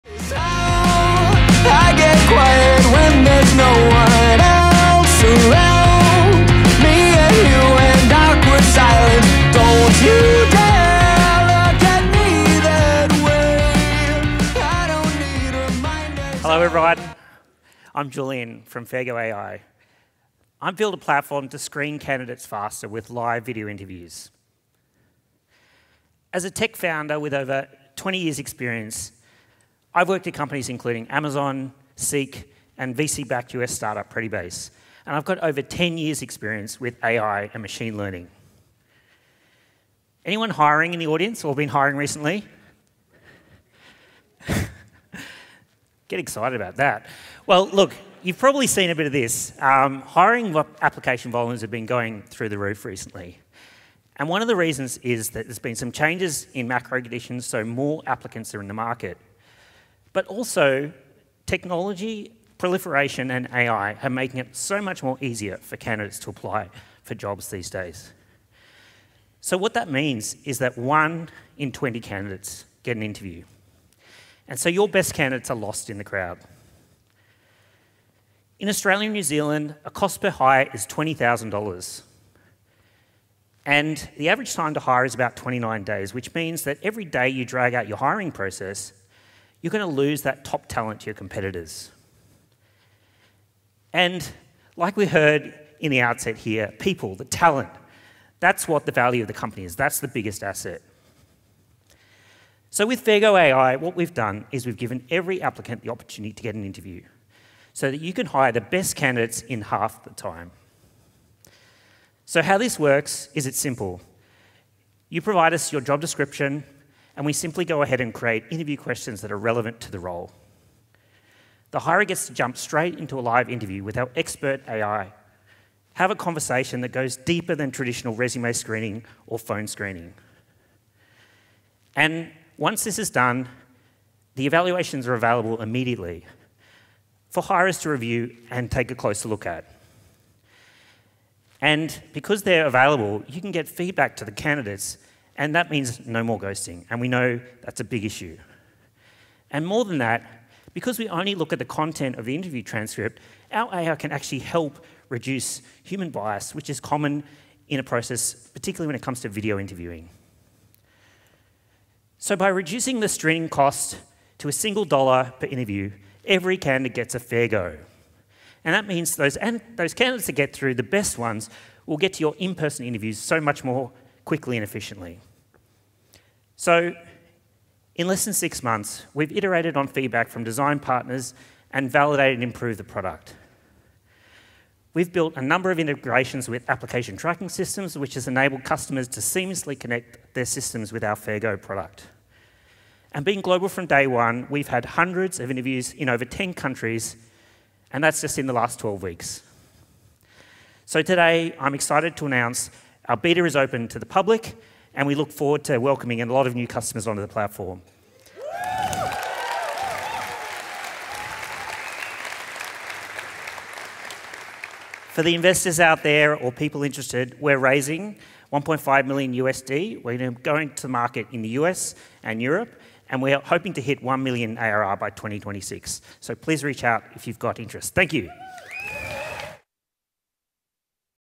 Faigo.ai
💥 Live, AI-powered recruitment interviews that cut through noise, eliminate bias, and find the best candidates, faster.

Fairgo.ai is an AI-powered live video interview platform that cuts the hiring time by 50% through automated screening. Unlike traditional methods, every question and response is not simply captured, but discussed with an AI expert. Candidate suitability is assessed and summarised, delivering in-depth insights for high-quality, efficient hiring decisions that minimises unconscious bias.

Founder: Julian Bright
Location: Melbourne, Australia